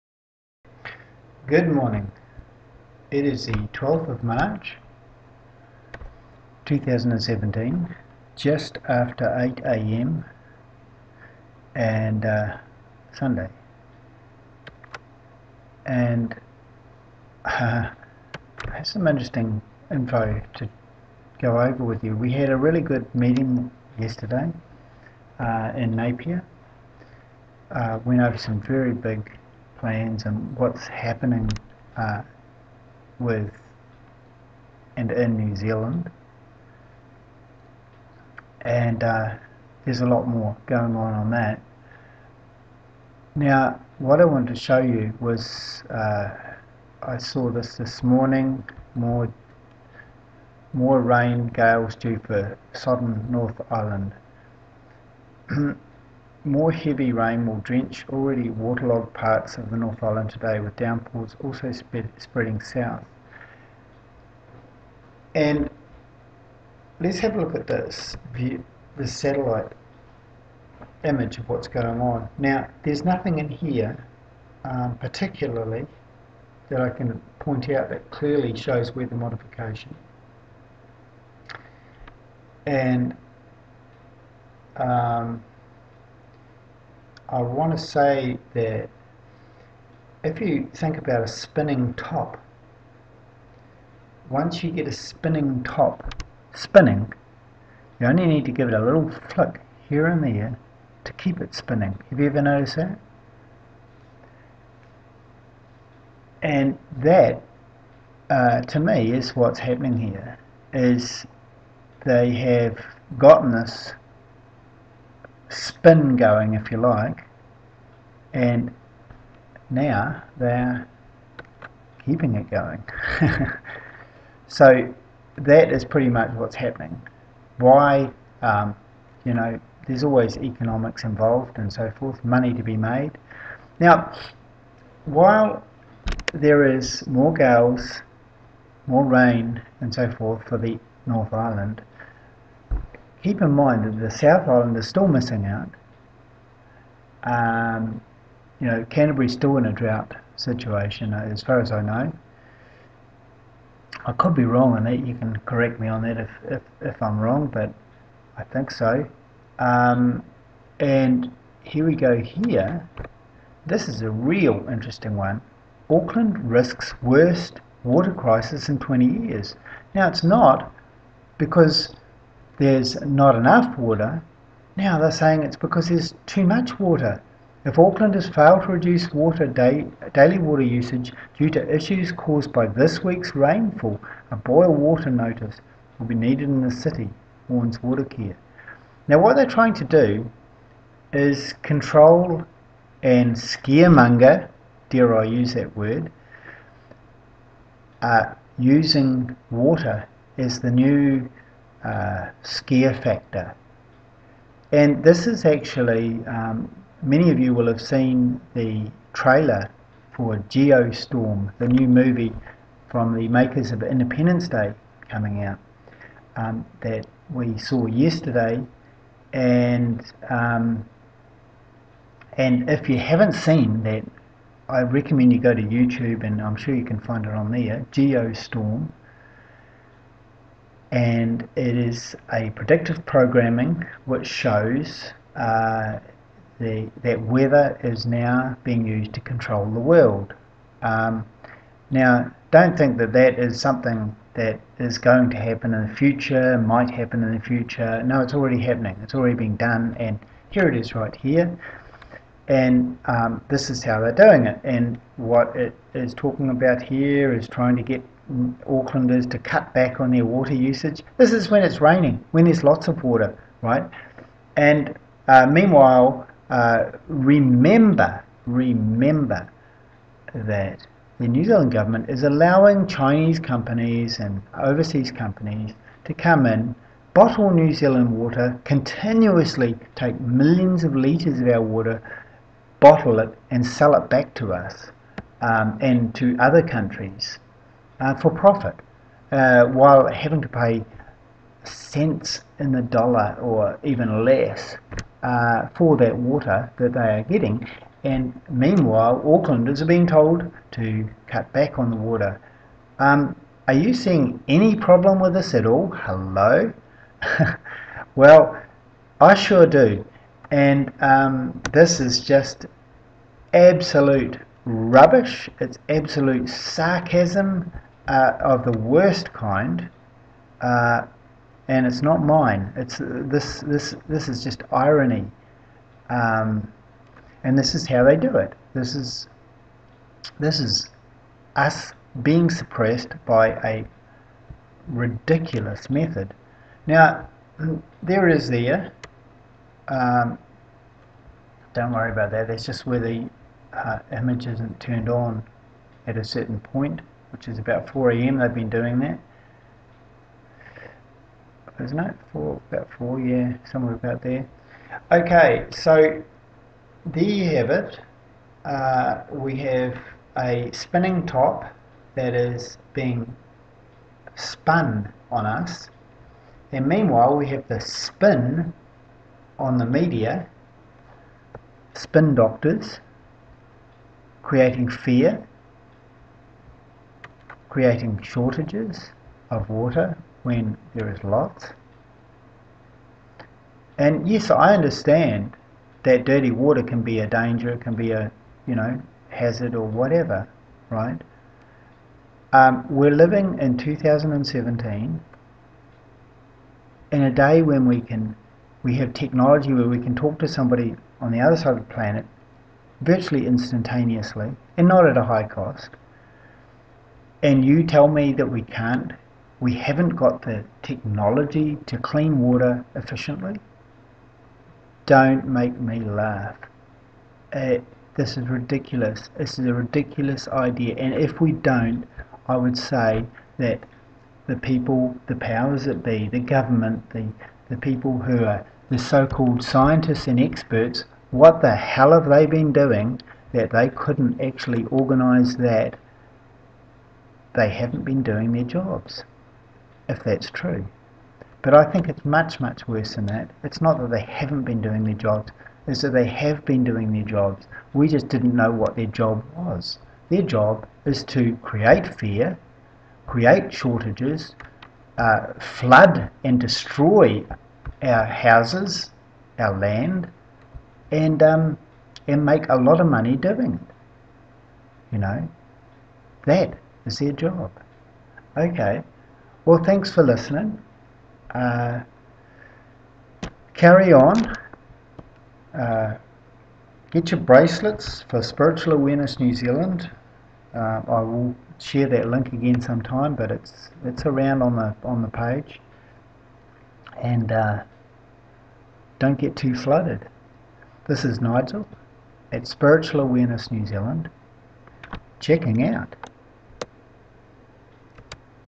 The Strategic Flooding of New Zealand 12 March 2017
The creation of a water crises is underway. Geostorm New Zealand is being used to create "water shortages"...yes! Water shortages!
Stay Safe New Zealand!